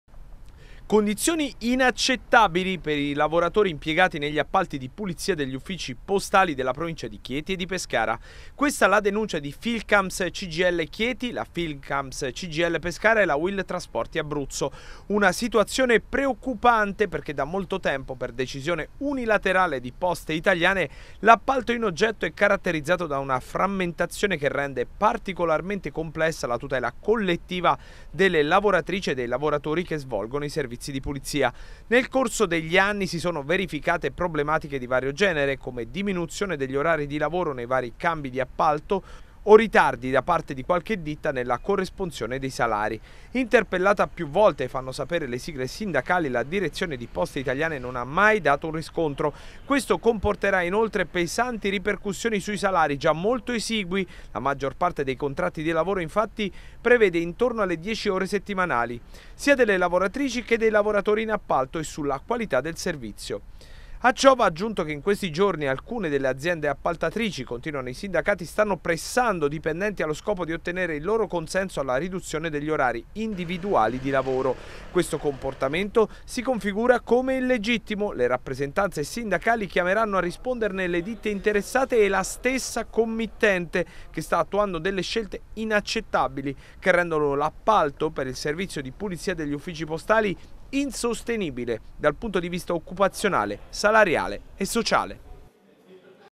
Appalti pulizia Poste, i sindacati: “Condizioni inaccettabili”
I sindacati Filcams Cgil di Chieti e Pescara e la Uil denunciano le condizioni di intollerabili per i lavoratori addetti alla pulizia degli uffici postali. L’azienda – ribadiscono – più volte interpellata, non ha mai fornito alcun riscontro